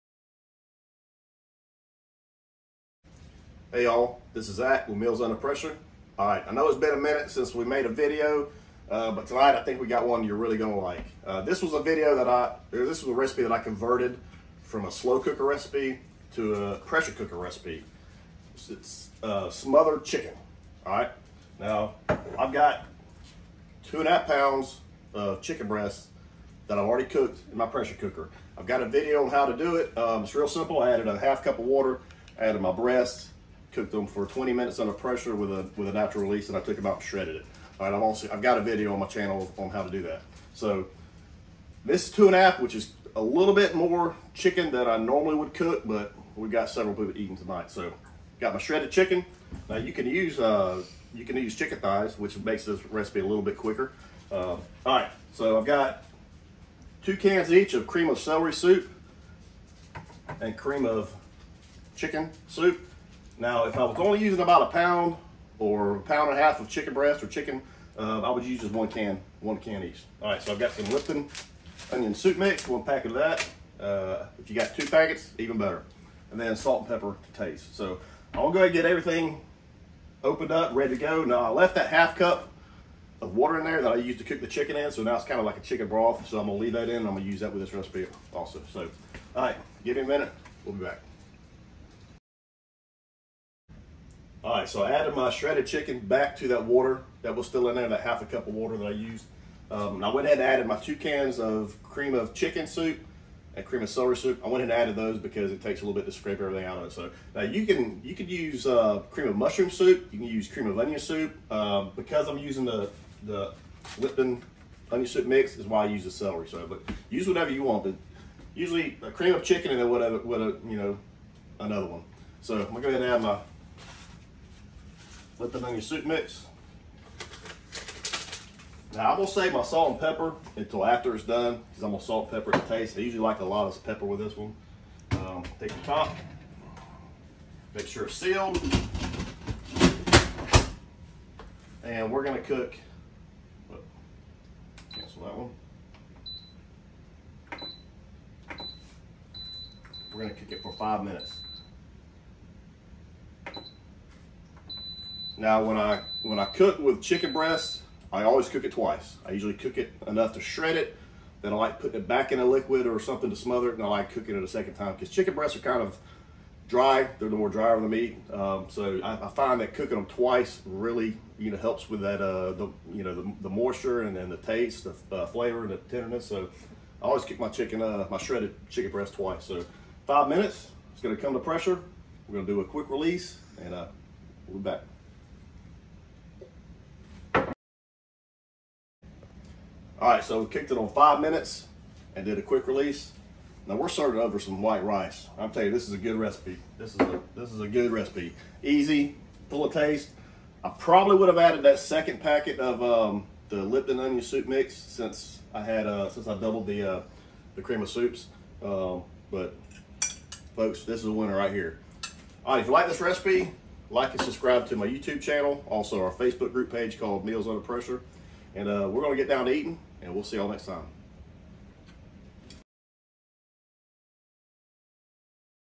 Smothered Chicken in the Instant Pot | Easy Recipe
This is an easy recipe that requires few ingredients.  Added on top of rice, it really goes a long way.  I always use Cream of Chicken Soup, but you can vary the other flavors of soup